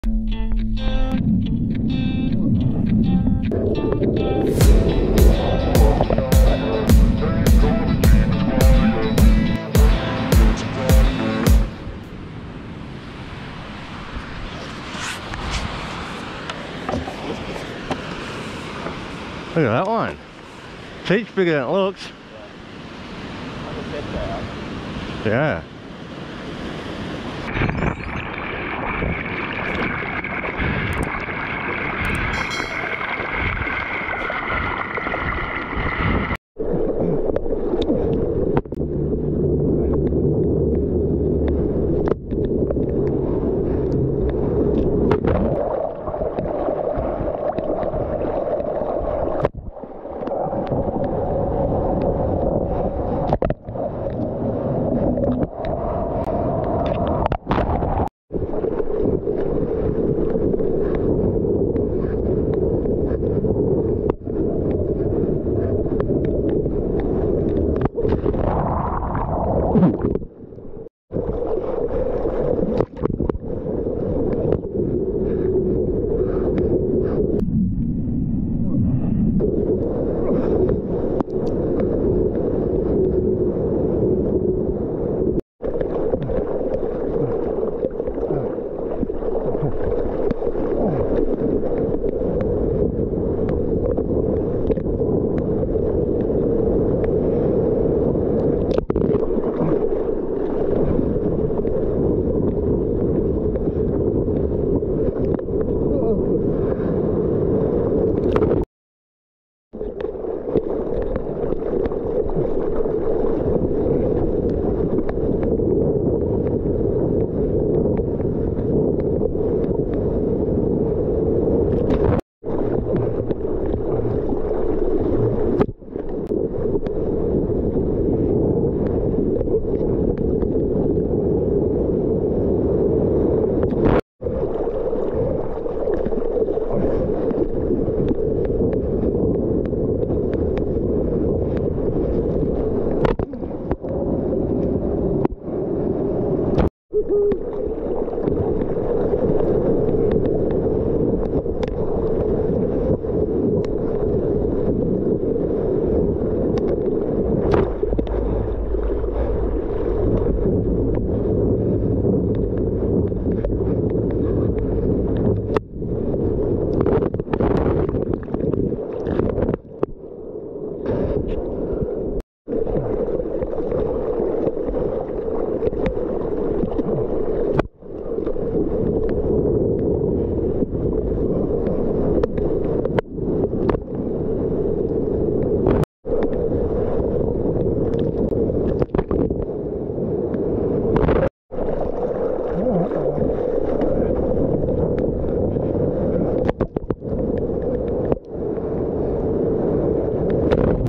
POV Surf | LIGHTENING FAST ⚡️JS SUB XERO Hyfi 2.0 First Test (raw)
I got a new surfboard and it's not a DHD Phoenix! I finally tried something new and it turns out it is an absolute weapon of a board. I think this could be the best small wave board I've ridden. Super fun conditions for testing this board, the type that it was built for. Small waves, fast little walls, turn sections, flat spots - everything. I hope you enjoy.

Gear used:
JS Industries Sub Xero Hyfi 2.0 5'9 35.3L
FCS II Mark Richards Twin Fin Plus Trailer (2 + 1)
FCS T3 Wide Tail Pad
FCS Helix 6ft Comp Leg Rope
GoPro 8 @ 2.7k 60fps Super View
Rip Curl Flash Bomb 3/2
O'Neil Hyperfreak Spring Suit
Quicksilver Spring Suit
FCS Cool water wax
Ho Stevie GoPro lanyard
Ho Stevie Surf Cap